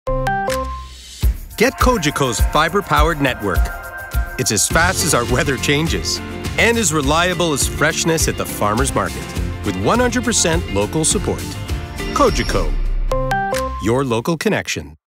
Kingston’s fast & reliable fibre-powered network
Cogeco delivers local support from A to Zed on a fast and reliable fibre-powered network. How fast and reliable? If you’re from Kingston, you can relate.